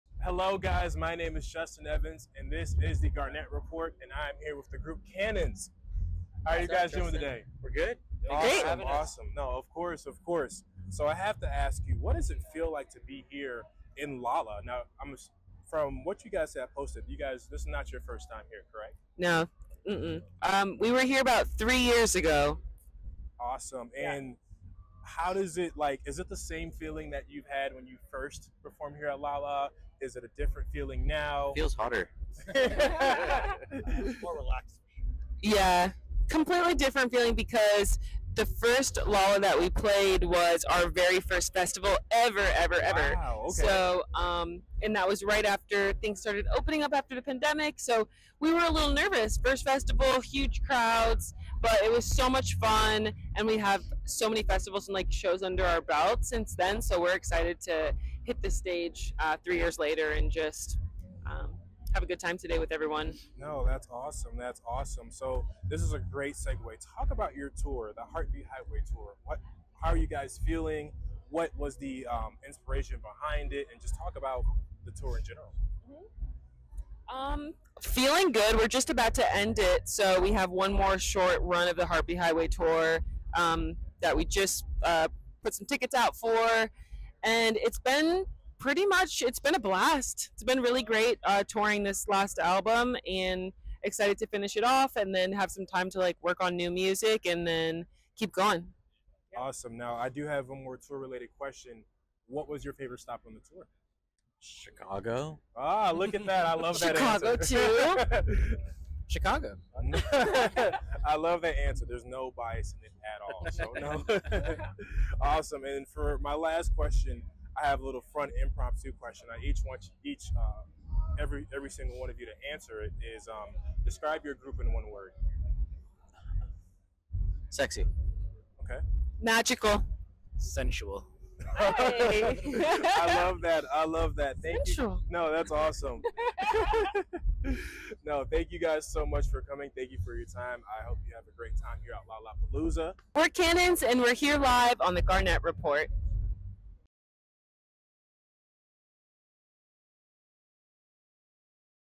Cannons Interview With TGR at Lollapalooza.
The Garnette Report had an opportunity to interview Cannon's at Lollapalooza's 2024 festival.